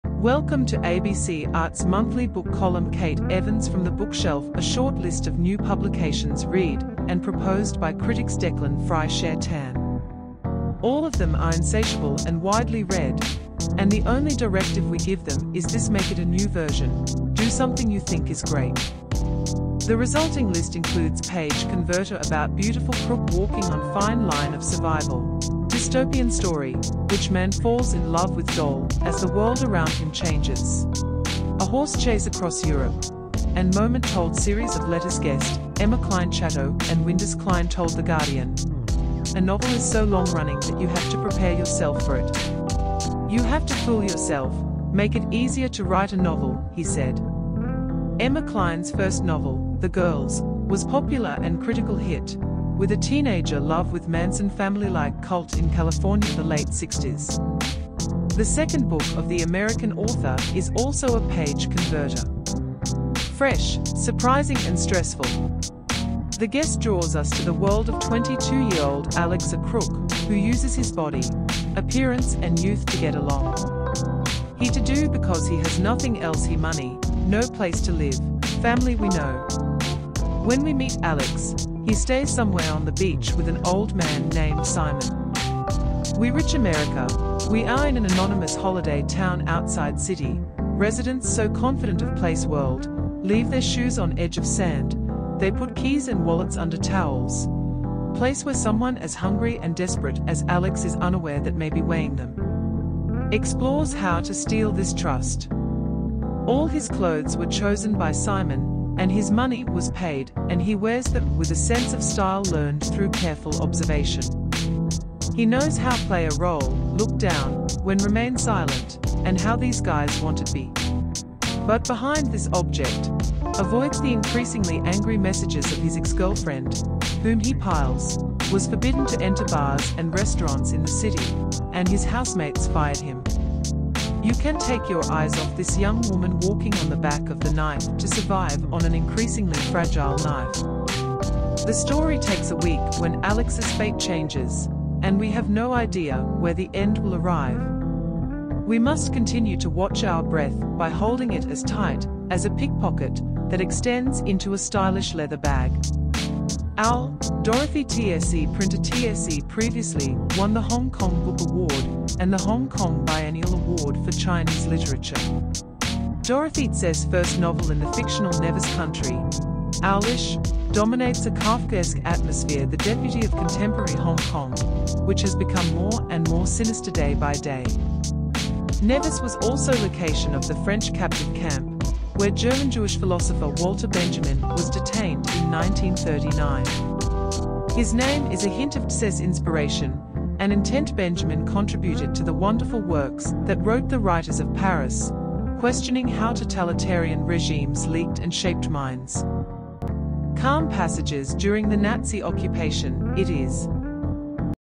The best new books released in May, as selected by avid readers and critics
Welcome to ABC Arts' monthly book column Kate Evans from The Bookshelf a short list of new publications read and proposed by critics Declan Fry Cher Tan. All of them are insatiable and widely read, and the only directive we give them is this make it a new version; do something you think is great. The resulting list includes page converter about beautiful crook walking on fine line of survival; dystopian story which man falls in love with doll as the world around him changes; A horse chase across Europe; and moment told series of letters  Guest, Emma Cline Chatto and Windus Cline told The Guardian, "A novel is so long-running that you have to prepare yourself for it. You have to fool yourself, make it easier to write a novel, "he said. Emma Cline's first novel, The Girls , was popular and critical hit, with a teenager love with Manson Family-like cult in California the late 60s. The second book of the American author is also a page converter. Fresh, surprising and stressful. The guest draws us to the world of 22-year-old Alex a crook who uses his body, appearance and youth to get along. He to do because he has nothing else he money, no place to live, family we know. When we meet Alex, he stays somewhere on the beach with an old man named Simon. We rich America, we are in an anonymous holiday town outside city; residents so confident of place world, leave their shoes on edge of sand, they put keys and wallets under towels, A place where someone as hungry and desperate as Alex is unaware that may be weighing them. explores how to steal this trust. All his clothes were chosen by Simon and his money was paid and he wears them with a sense of style learned through careful observation. He knows how play a role, look down, when remain silent, and how these guys want it be. But behind this object, avoids the increasingly angry messages of his ex-girlfriend, whom he piles; was forbidden to enter bars and restaurants in the city; and his housemates fired him. You can't take your eyes off this young woman walking on the back of the knife to survive on an increasingly fragile knife. The story takes a week when Alex's fate changes, and we have no idea where the end will arrive. We must continue to watch our breath by holding it as tight as a pickpocket that extends into a stylish leather bag. Owl, Dorothy Tse printer Tse previously won the Hong Kong Book Award and the Hong Kong Biennial Award for Chinese Literature. Dorothy Tse's first novel in the fictional Nevers country, Owlish, dominates a Kafkaesque atmosphere - the deputy of contemporary Hong Kong, which has become more and more sinister day by day. Nevers was also location of the French captive camp, where German-Jewish philosopher Walter Benjamin was detained in 1939. His name is a hint of Tse's inspiration and intent Benjamin contributed to the wonderful works that wrote the writers of Paris, questioning how totalitarian regimes leaked and shaped minds. Calm passages during the Nazi occupation, "it is